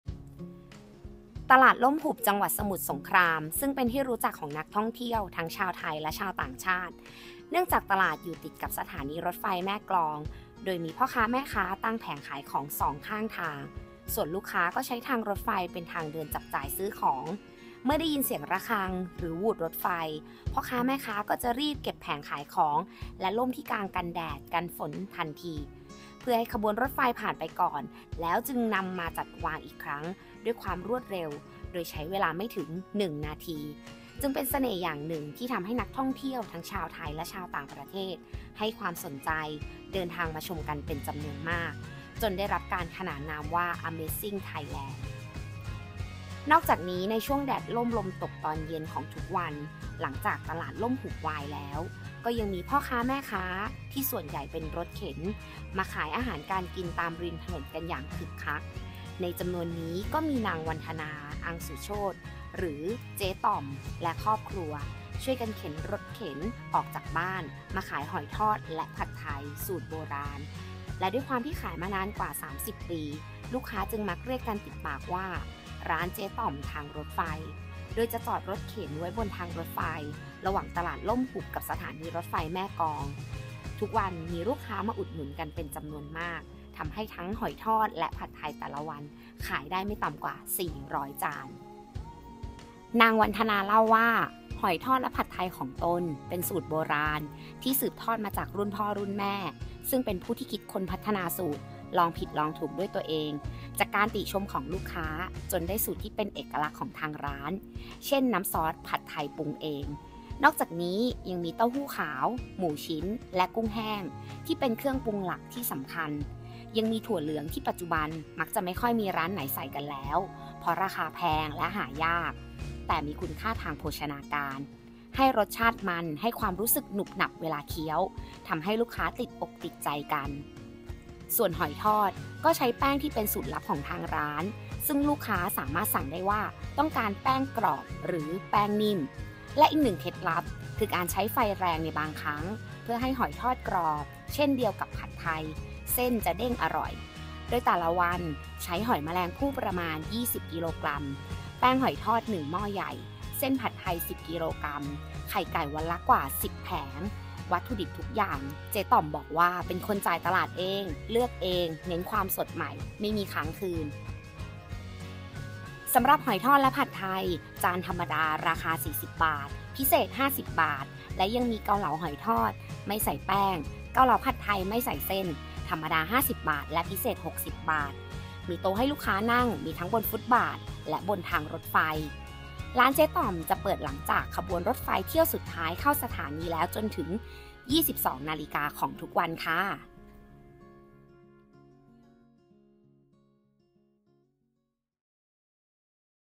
หนึ่งเดี๋ยวในโลก หอยทอดนั่งกินบนรางรถไฟ ที่ตลาดร่มหุบแม่กลอง : เรื่องเด่นทั่วไทย
หนึ่งเดี๋ยวในโลก “ร้านเจ๊ต่อม หอยทอดผัดไทยสูตรโบราณ” นั่งกินบนรางรถไฟ ที่ตลาดร่มหุบแม่กลอง  มานานกว่า 30 ปี ขายดิบขายดีวันละกว่า 400 จาน ใครต้องการรับประทานไปชิมกันได้หลังขบวนรถไฟเที่ยวสุดท้ายเข้าสถานี

เจ๊ต่อม เล่าว่าหอยทอดและผัดไทยของตนเป็นสูตรโบราณที่สืบทอดมาจากรุ่นพ่อรุ่นแม่ ซึ่งเป็นผู้ที่คิดค้นพัฒนาสูตร ลองผิดลองถูกด้วยตัวเองจากการติชมของลูกค้าจนได้สูตรที่เป็นเอกลักษณ์ของทางร้าน